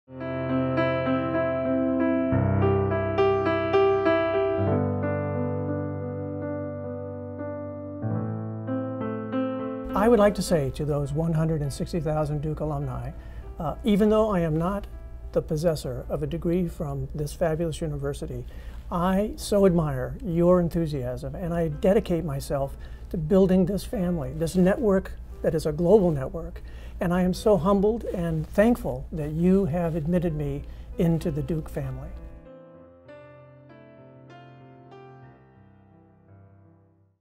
Duke's President-elect Shares a Message with Alumni
Vincent Price, president-elect of Duke, delivers a message to more than 160,000 alumni.